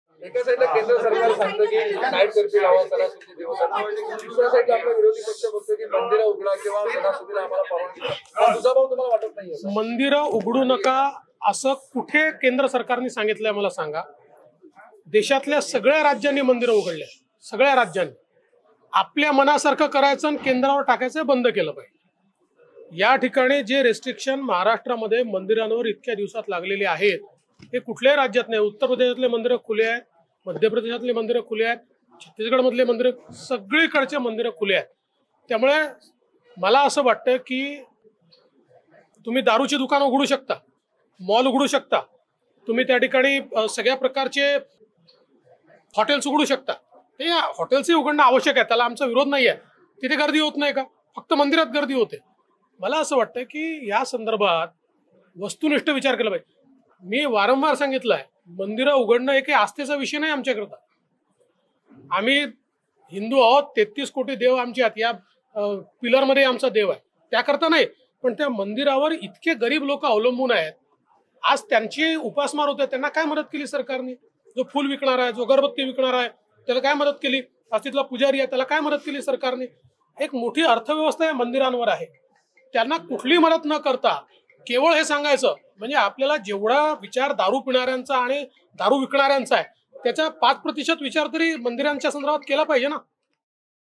राज्य सरकारने मद्यपेयी-विक्रेत्यांचा जसा विचार केला तसा मंदिरांवर उदरनिर्वाह करणार्‍यांचाही करावा!
मंदिरं खुली करण्यावरून आपले अपयश लपविण्यासाठी केंद्र सरकारवर दोषारोपण बंद करा.
इतर राज्यांत मंदिरं खुली झाली आहेत! नवी मुंबई येथे माध्यमांशी संवाद..| १ सप्टेंबर २०२१

मंदिरे उघडू नका हे कोणी सांगितले?

केंद्र सरकारने मंदिरे उघडू नका असे कुठेही सांगितलेले नाही. राज्य सरकारने आपल्या मनासारखे करायचे आणि केंद्र सरकारवर ढकलायचे हे बंद केले पाहिजे. महाराष्ट्रात मंदिरांवर जे निर्बंध लागले आहेत ते कोणत्याही राज्यात नाहीत. इतर राज्यांमध्ये मंदिरे खुली. राज्य सरकार दारुची दुकाने, मॉल्स, हॉटेल्स उघडायला परवानगी देते. हॉटेल्स उघडायला हरकत नाही, मात्र तिथे गर्दी होत नाही का? फक्त मंदिरातच गर्दी होते का? आम्ही हिंदू, आमचे 33 कोटी देव आहेत. फक्त आस्थेसाठी नाही तर मंदिरांवर अनेक गरीब लोक अवलंबून. त्यांची उपासमार होत असताना राज्य सरकारने त्यांना काय मदत केली? फूल विक्रेते, अगरबत्ती विक्रेते, प्रसाद विक्रेते यांना राज्य सरकारने कोणतीही मदत केलेली नाही. मंदिरावर मोठी अर्थव्यवस्था अवलंबून आहे. जेवढा विचार मद्यपेयी आणि विक्रेत्यांचा करण्यात आला त्या तुलनेत किमान 5% तरी मंदिरांचा विचार राज्य सरकारने केला पाहिजे.